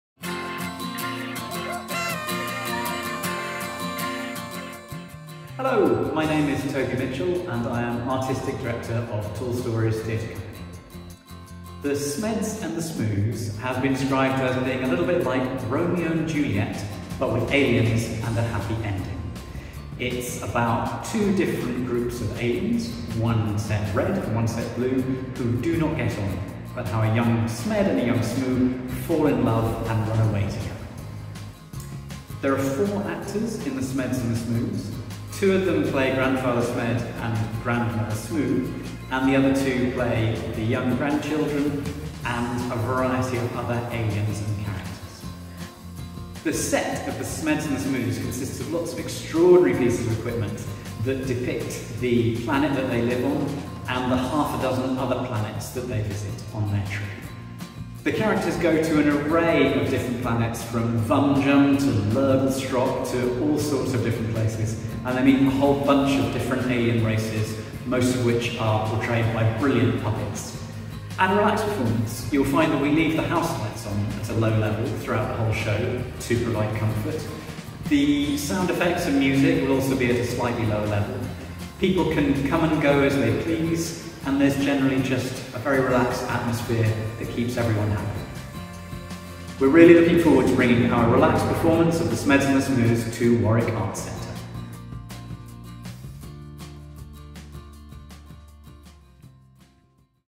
The Smeds and The Smoos Relaxed Performance
Hear from Artistic Director of The Smeds and The Smoos, Toby Mitchell, about what you can expect from our relaxed performance on Friday 2 June at 11am.